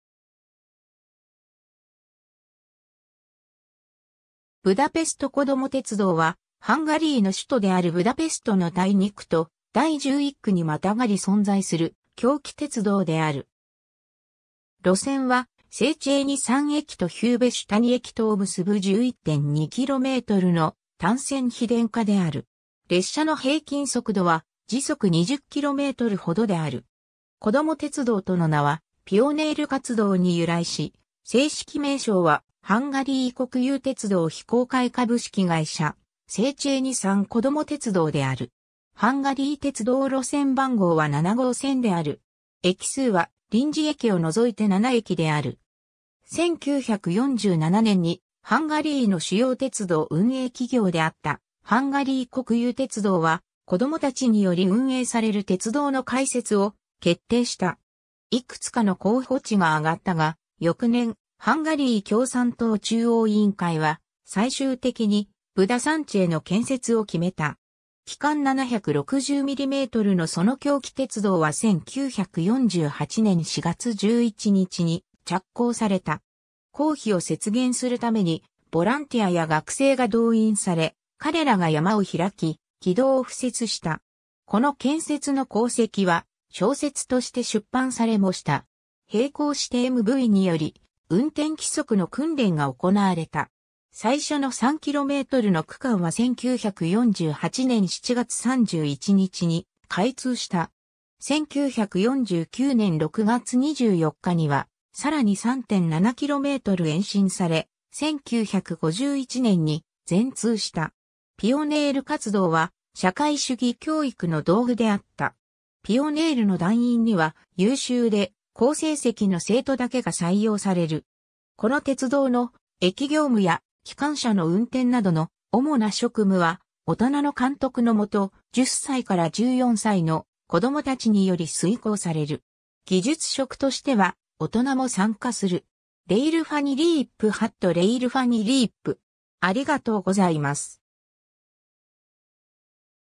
ブダペスト子供鉄道
ブダペスト子供鉄道は、ハンガリーの首都であるブダペストの第2区と第11区にまたがり存在する狭軌鉄道である。
路線はセーチェーニ山（セーチェーニヘジュ）駅とヒューヴェシュ谷（ヒューヴェシュヴェルジュ）駅とを結ぶ11.2kmの単線非電化である。
列車の平均速度は20km/hほどである。
 子供鉄道との名は、ピオネール活動に由来し、正式名称は「ハンガリー国有鉄道非公開株式会社セーチェーニ山子ども鉄道」(MÁV Zrt. Széchenyi-hegyi Gyermekvasút)である。
ハンガリー鉄道路線番号は7号線 (7-es számú vasútvonal) である。
駅数は臨時駅を除いて7駅である。
 1947年に、ハンガリーの主要鉄道運営企業であったハンガリー国有鉄道 (MÁV) は、子供たちにより運営される鉄道の開設を決定した。
いくつかの候補地が挙がったが、翌年、ハンガリー共産党中央委員会は最終的にブダ山地への建設を決めた。
 軌間760mmのその狭軌鉄道は1948年4月11日に着工された。
工費を節減するために、ボランティアや学生が動員され、彼らが山を開き、軌道を敷設した。
この建設の功績は小説として出版されもした。
並行してMÁVにより運転規則の訓練がおこなわれた。
 最初の3kmの区間は1948年7月31日に開通した。
1949年6月24日にはさらに3.7km延伸され、1951年に全通した。
 ピオネール活動は、社会主義教育の道具であった。
ピオネールの団員には、優秀で好成績の生徒だけが採用される。
 この鉄道の、駅業務や機関車の運転などの主な職務は、大人の監督のもと、10歳から14歳の子供たちにより遂行される。
技術職としては大人も参加する。
 ^ RailfanEurope （英語） ^ RailfanEurope （英語）